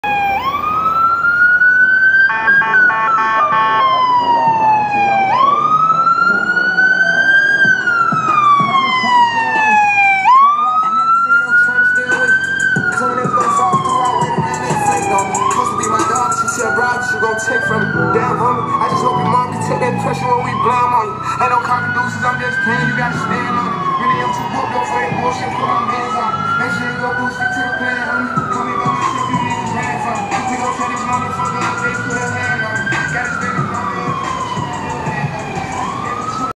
American Medical Response Ambulance 48 Responding
AMR ambulance 48 responding to an unknown medical call. Notice the jackass following the ambulance. Take this video as a prime example of what not to do when an emergency vehicle is responding.

Date: 10/27/23
Call Type: Unknown
Camera information: Canon Vixia
Vehicle Information:2015 Ford E-350/AEV